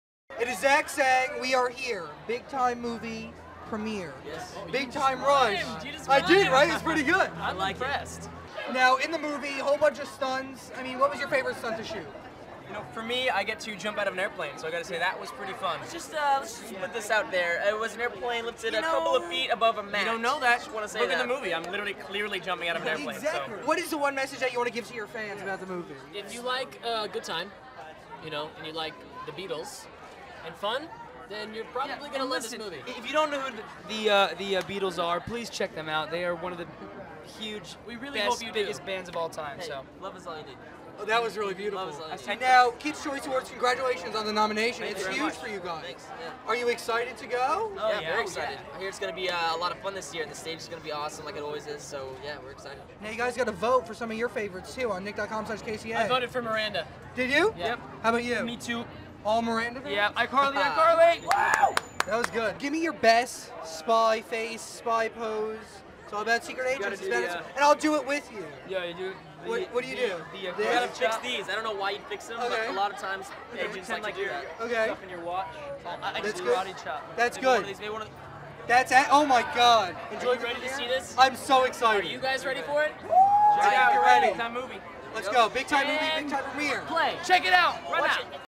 Big Time Movie Premiere: BTR Takes the Orange Carpet!
It's official, the BTR guys are big time movie stars! Zach Sang caught up with them at the premiere of their movie in New York!